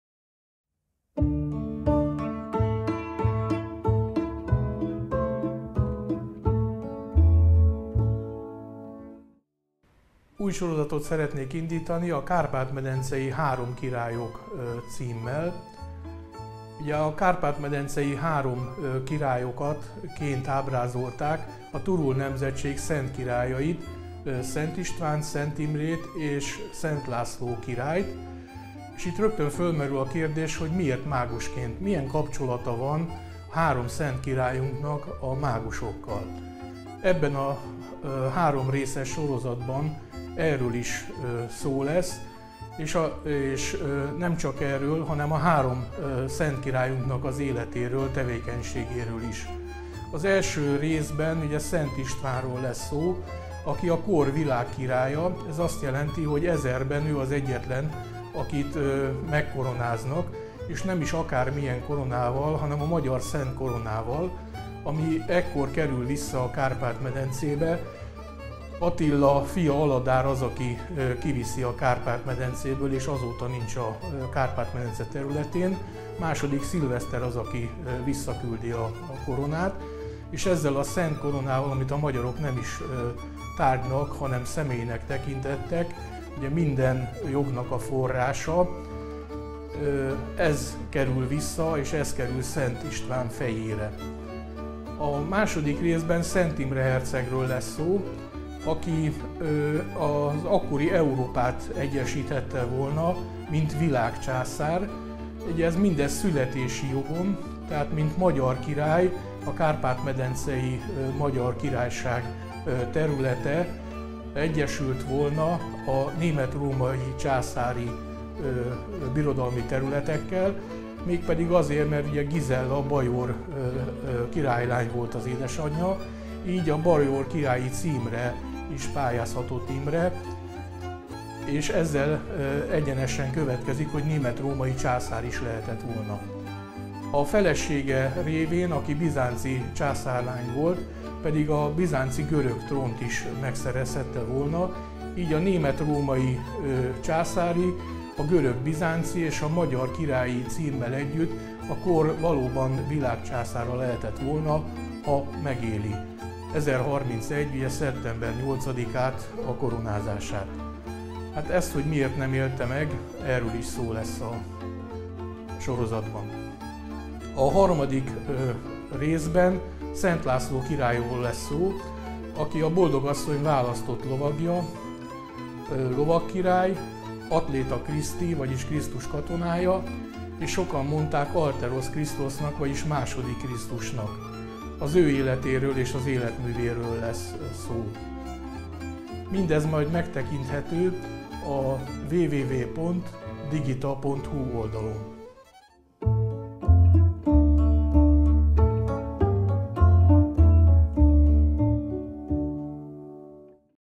ISMERTETŐ-Kárpát-medencei három királyok sorozat
Kiss Maotun István előadása elérhető oldalon

Mit is tartalmaznak a publikációk?
Részekre tagolt 1-1,5 órás videó előadások , hangoskönyv funkció és képgaléria az
előadásban megjelenő képekkel.
A Kárpát-medencei három királyok sorozatban a nimródi szent királyok, vagy másképpen a
Turul dinasztia három királyként ábrázolt szent királyait: Istvánt, Imrét és Lászlót mutatjuk
be. A Kárpát- medencei középkori templomainkban előszeretettel ábrázolták három
mágusként három szent királyunkat. Felmerül a kérdés jogosan, hogy miért, milyen
összefüggés van a három napkeleti mágus és a három szent királyunk között? Megtudhatjuk
azt is, hogy miért szent a Magyar Szent Korona és mért tekintették Élő Személynek és az
Utolsó Ítélet Krisztusi koronájának. Szent István az egyedüli király, akit Kr.u. 1000-ben
koronáztak meg a Kárpát-medencébe visszakerült Magyar Szent Koronával, Világ Királynak.
Szent Imre herceg koronázása 1031. szeptember 8-án lett volna, ha nem gyilkoltatja meg a
bajor uralkodó ház liudolf-i ágából való II. Konrád császár. Szent Imre világ császárrá
koronázása egyesítette volna az akkori Európát a Kárpát-medencei nimródi szent királyok
nemzettsége alatt, mivel Imre herceg születési jogán magyar király és bajor királyként a
német-római császári cím várományosa, házassága révén (felesége a bizánci császár leánya)
jogot formálhatott a bizánci trónra is. Szent László királyunk, mint a Boldogasszony
választott lovagja, Krisztus Atlétája és sokan mondták második Krisztusnak is, nem véletlen
hogy 1093-ban Bodrog várában, IV. Vilmos toulouse-i gróf felkéri a Keresztes hadak
vezérének László királyt.